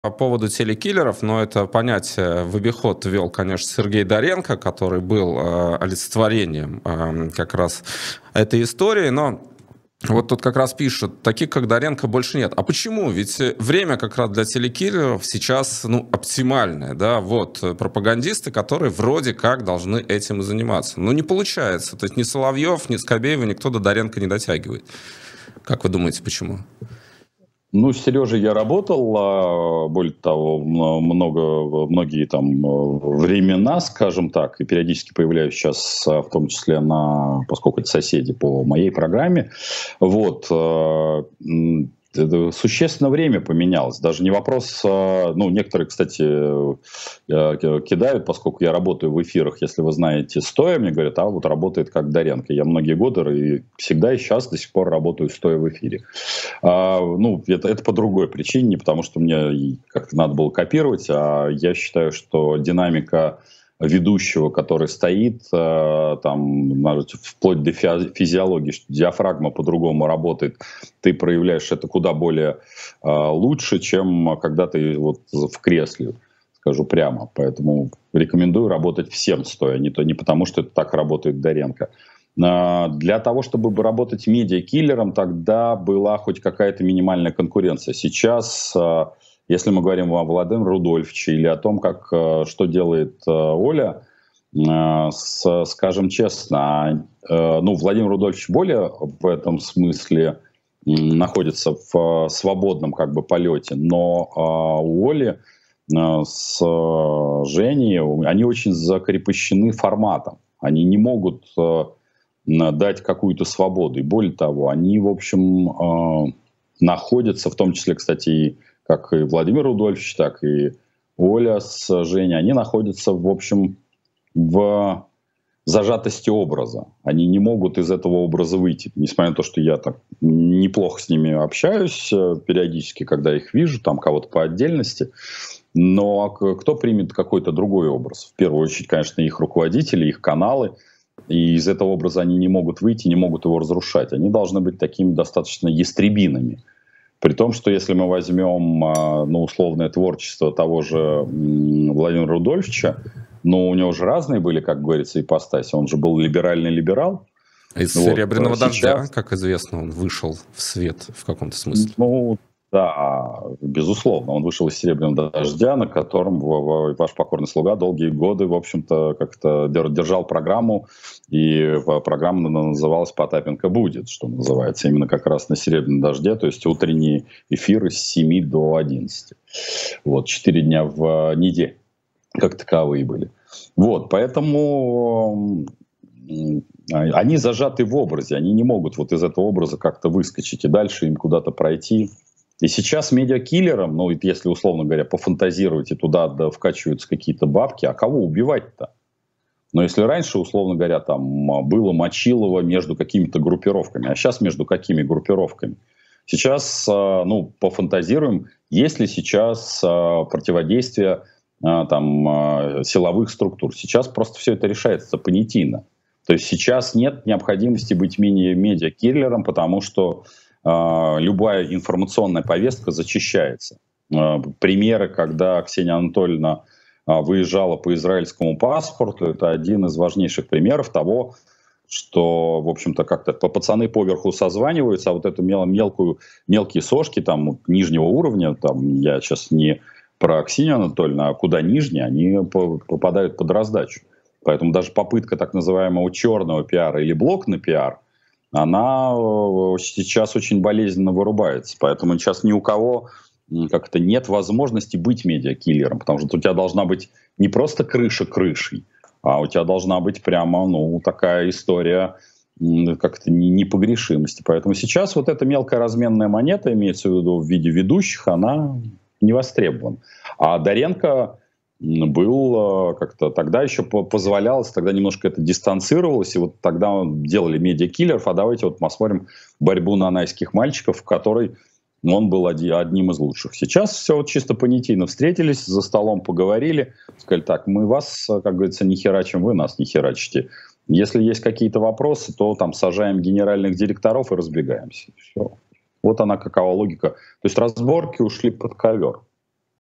Потапенко: почему сейчас нет «телекиллеров», похожих на Доренко? (15.01.2024)
из разговора 15 января 2024 г.
взято с канала "И грянул Грэм"